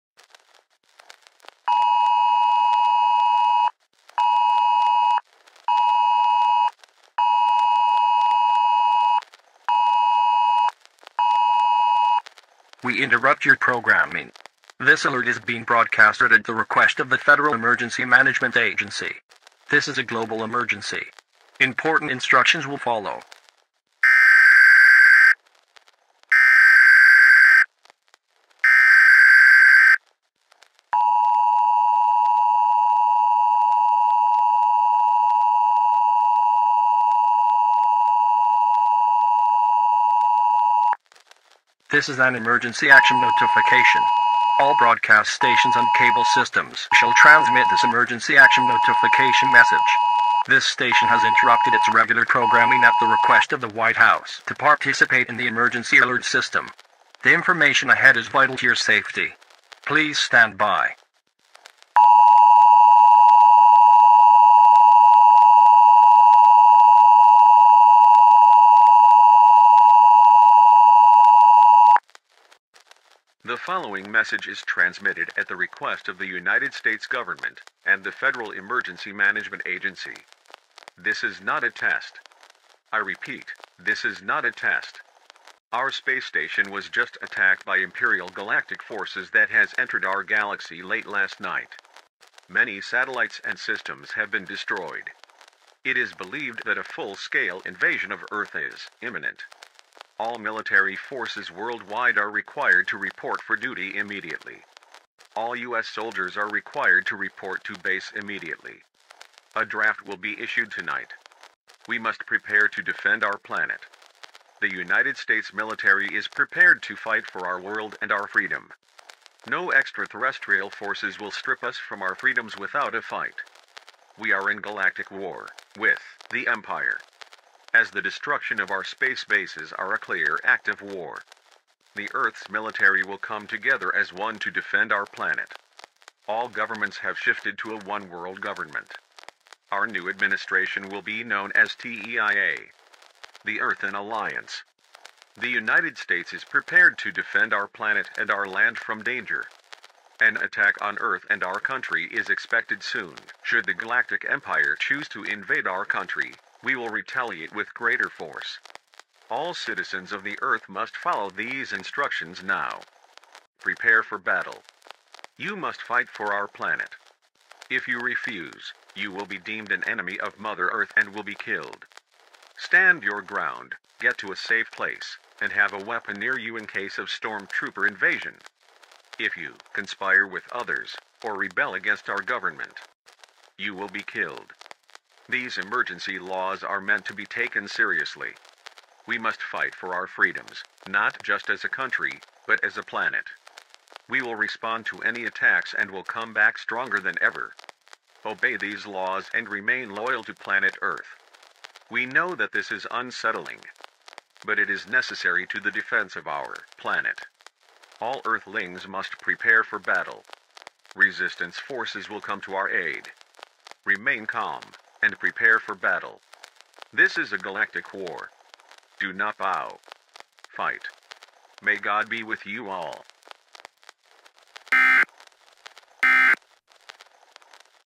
T. E. I. A [EAS MOCK]
These are VALID TONES. Do NOT play these over the air I (FT Scenarios) am NOT responsible for any activations of the EAS due to misuse of the video.
We take the EAS Very seriously as we should.
I just enjoy making EAS Scenarios for people to enjoy.